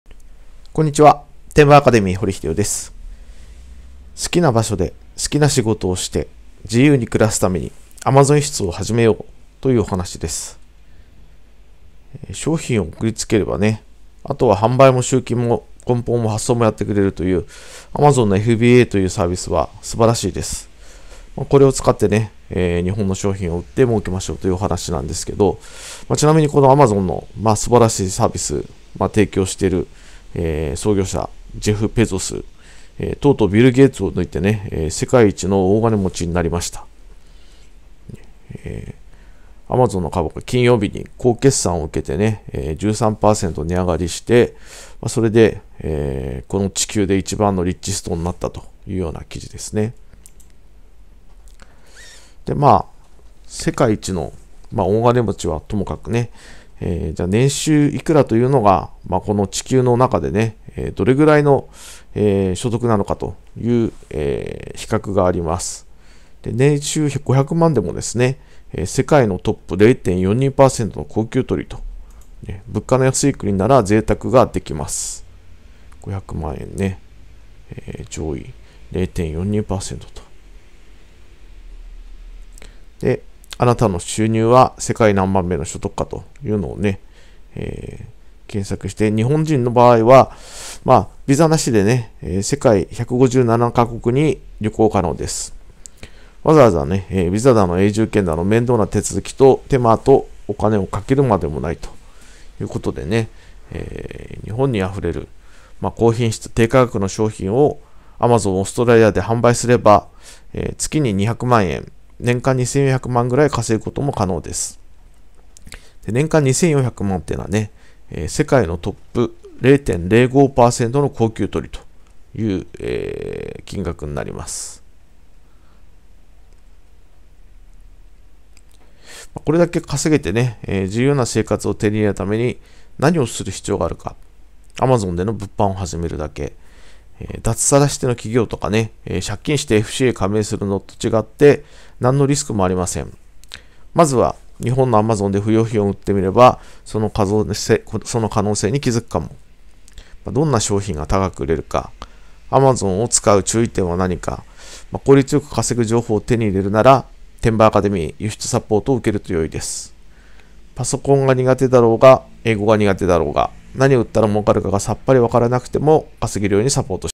好きな場所で好きな仕事をして自由に暮らすためにAmazon輸出をはじめよう
実はとても住みにくい国、日本。外国人が働きたい国ランキング日本は全ての項目において評価が低い。賃金については最下位。ワークライフバランスについても最下位。子どもの教育環境についても最下位。日本人が海外で「出稼ぎ」する未来？
掘　英郎 Hideo Hori アマゾン輸出転売アカデミー公式
←メルマガ登録して下さい。

1日10分で月10万円を稼ぐ! Amazon輸出ビジネスがわかる本　出版社:ソシム (2013/11/22)